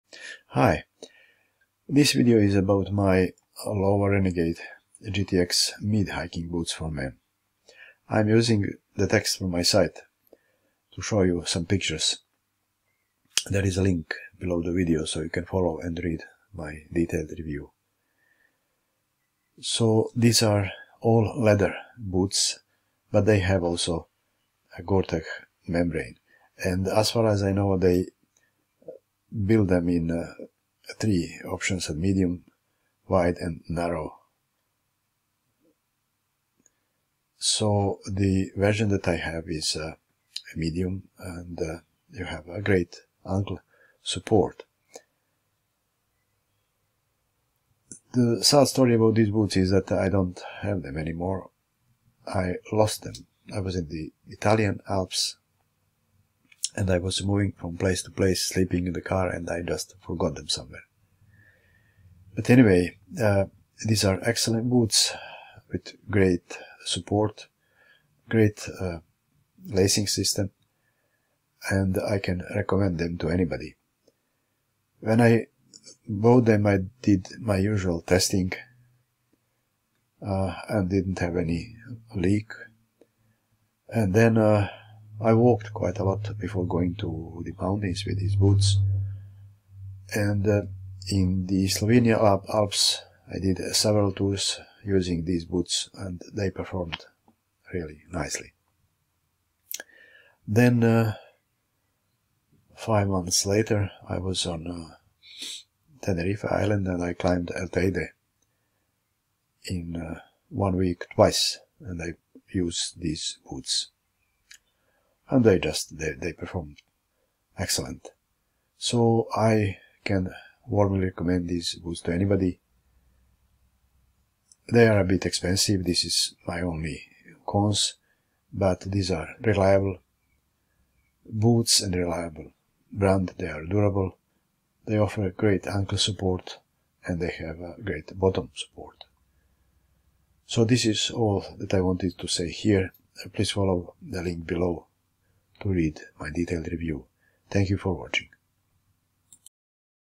Lowa Renegade GTX Mid Hiking Boots for Men Review
I needed new boots and this Lowa Renegade GTX Mid Hiking Boots for Men review is about my new pair. They are comfortable out of the box, a bit surprising for all-leather boots.